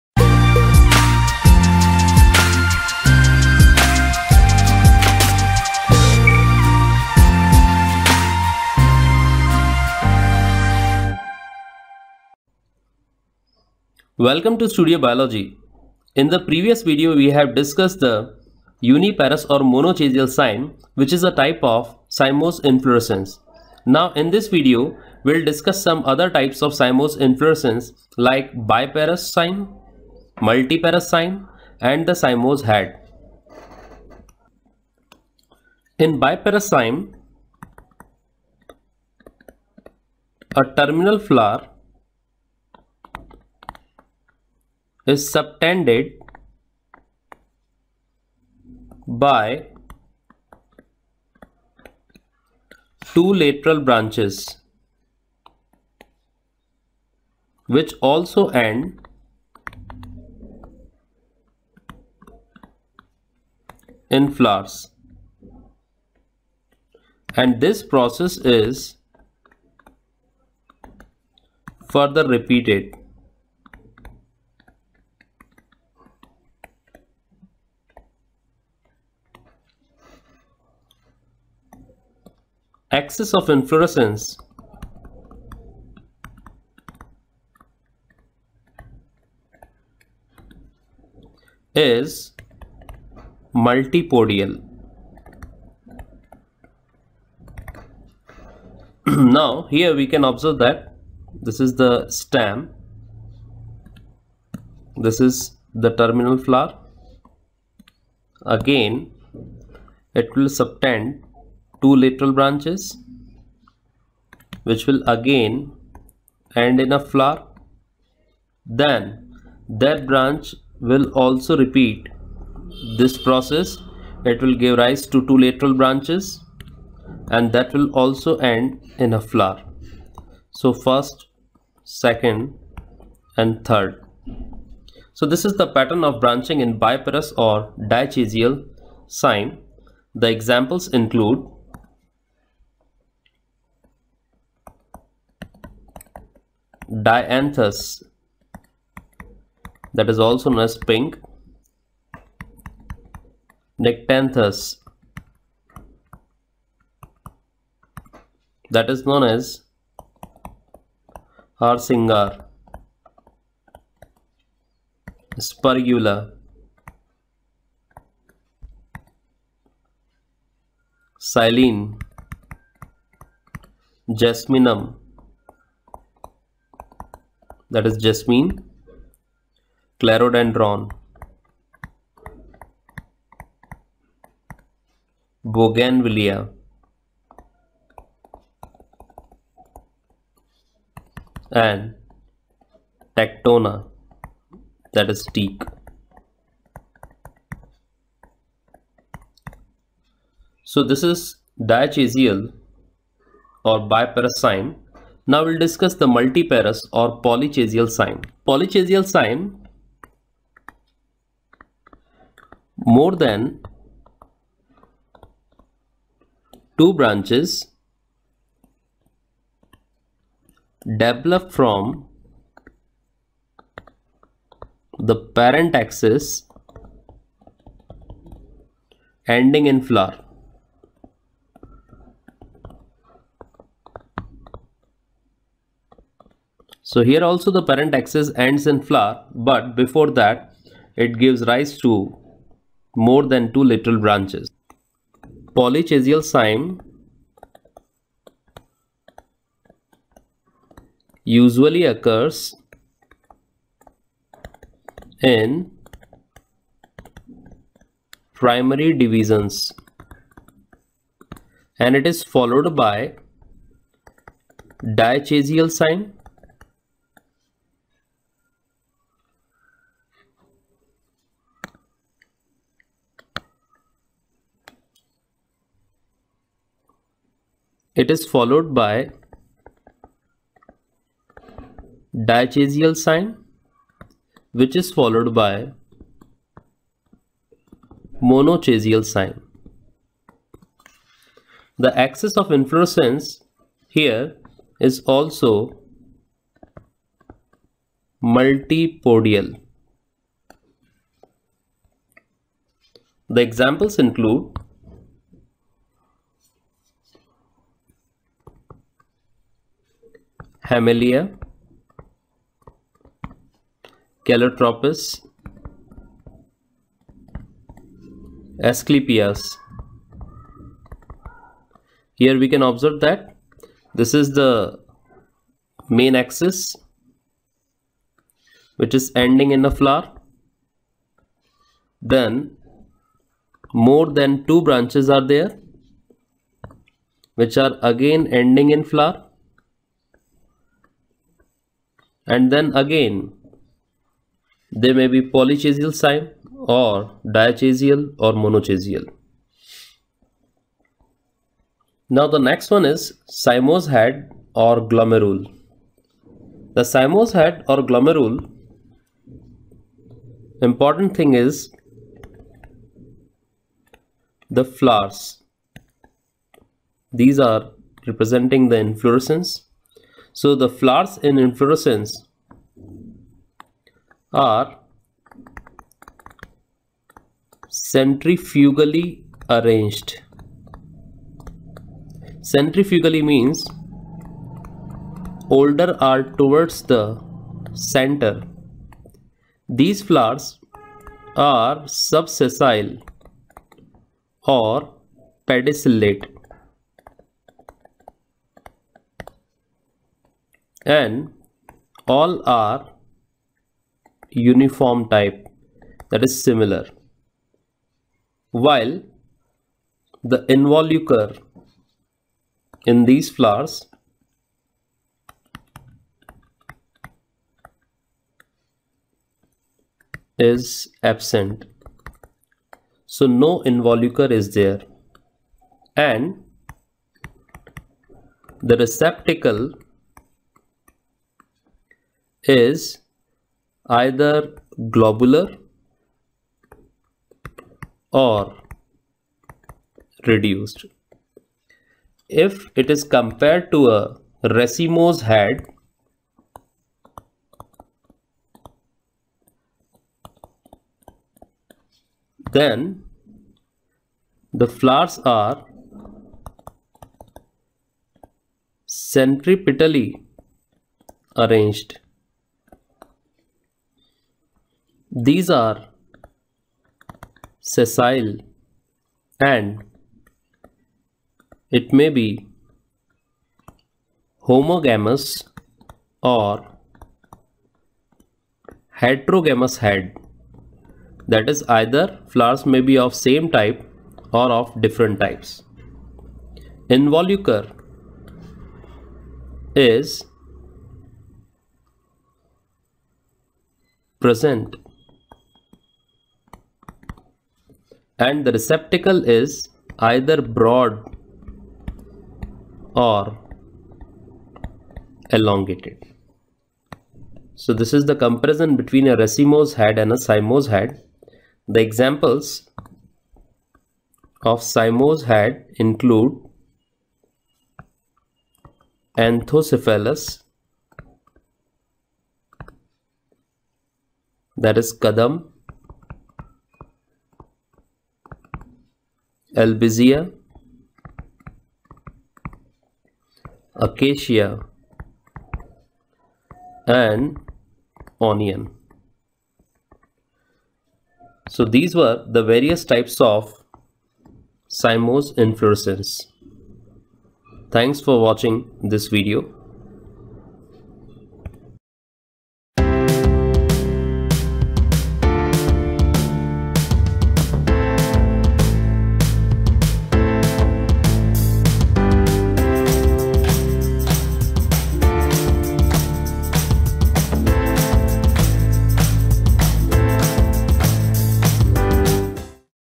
BOTANY XI.05.080 – Morphology of Angiosperms – Dichasial, Polychasial, Cyathium Inflorescence
In this video on Morphology of Angiosperms, Dichasial, Polychasial and Cyathium Inflorescence are discussed.